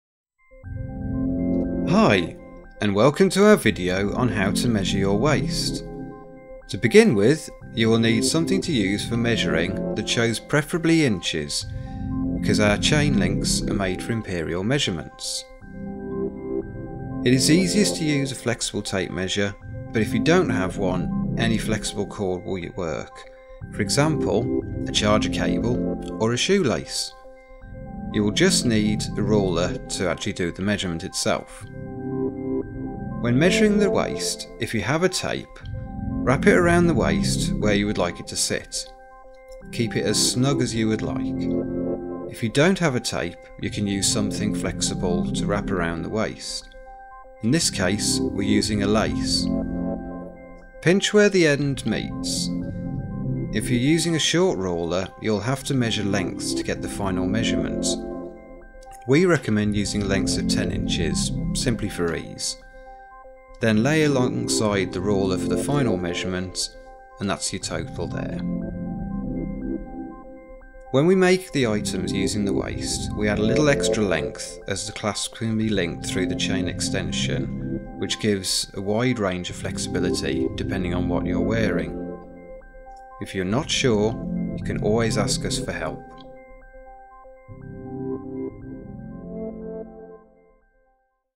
How to Measure Your Waist
Handmade to order in the UK so please select your waist size when you order if you are between sizes let us know in the order notes and we will make it to your exact size where possible. To measure your waist place the tape measure around your waist where you would like the item to sit. Don't measure too tightly or add extra to your measurements (we do that for you) as it will effect the fit of your item if your aiming for a tight fit.